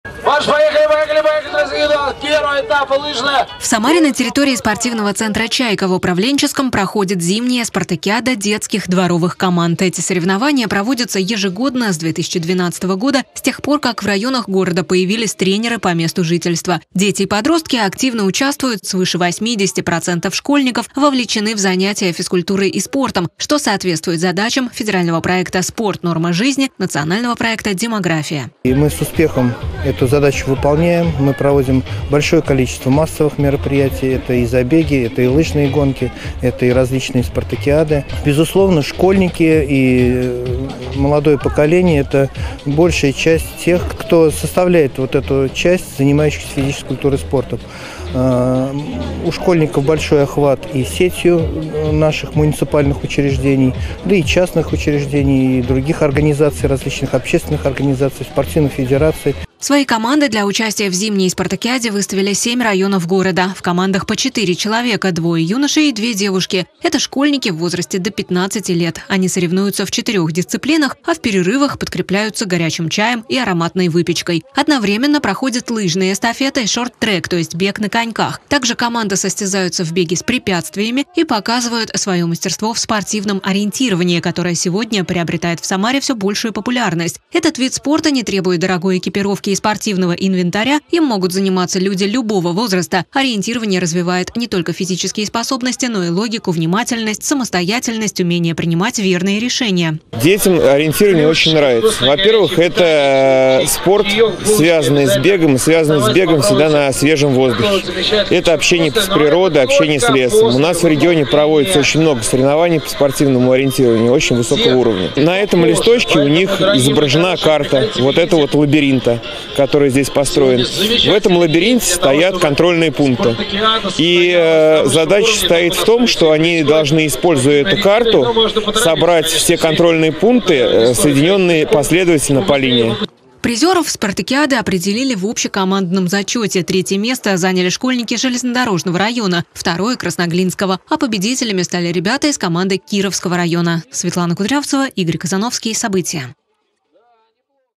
В Самаре прошла зимняя спартакиада детских дворовых команд
Соревновались в беге на лыжах и на коньках, в спортивном ориентировании и беге с препятствиями. В Самаре состоялась зимняя спартакиада детских дворовых команд. Мы побывали на соревнованиях и готовы поделиться подробностям.